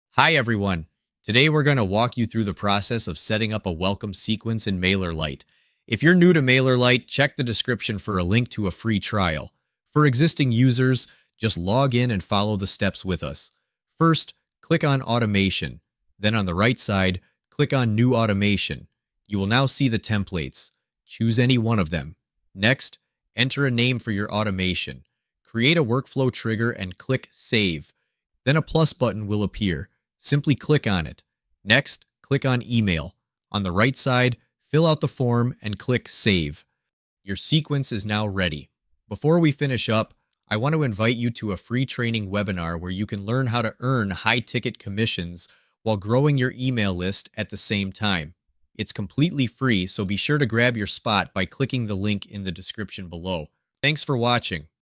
How to Create a Welcome Email Sequence on MailerLite (Easily)
How to Create a Welcome Email Sequence on MailerLite

In this video, we’ll guide you through setting up a welcome sequence in MailerLite. Whether you’re new or an existing user, follow these steps to create a seamless welcome automation for your subscribers.

Create a welcome sequence in MailerLite
Set up automation and email triggers
Boost your email list effectively